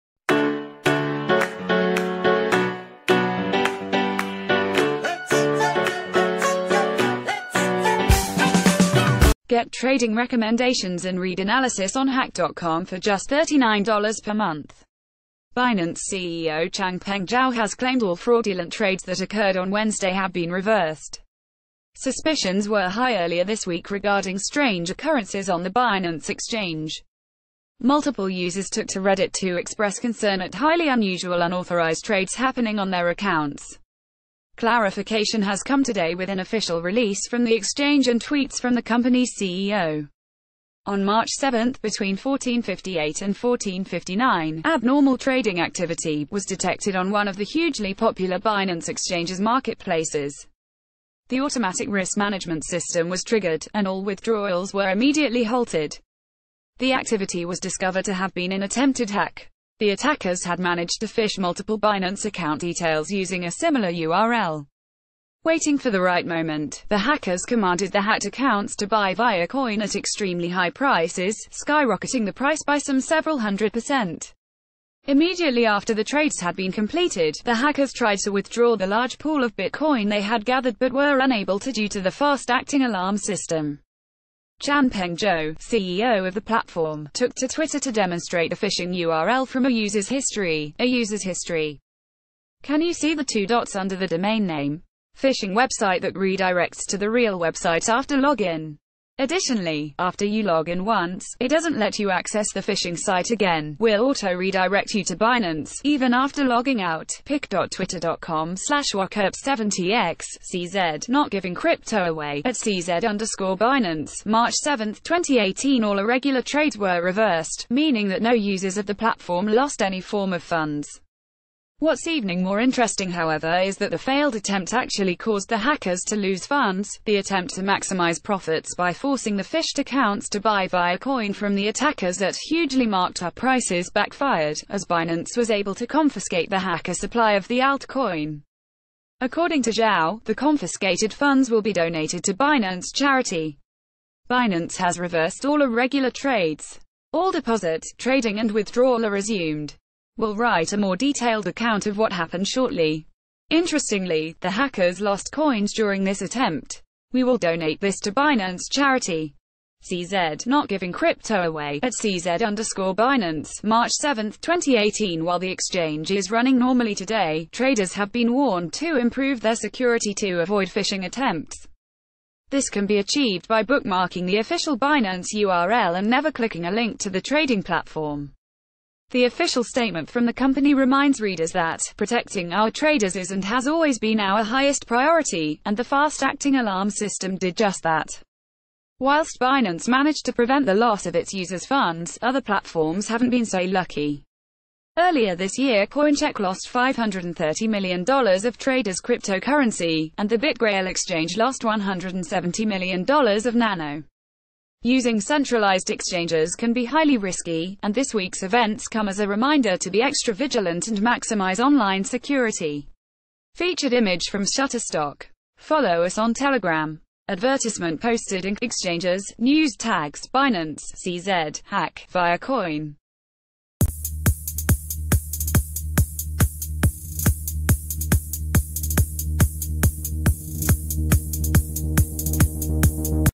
Binance CEO: All Irregular Trades Reversed, Hackers Lost Coins
Get Trading Recommendations and Read Analysis on Hacked.com for just $39 per month. Binance CEO Changpeng Zhao has claimed all fraudulent trades that occurred on Wednesday have been reversed. Suspicio...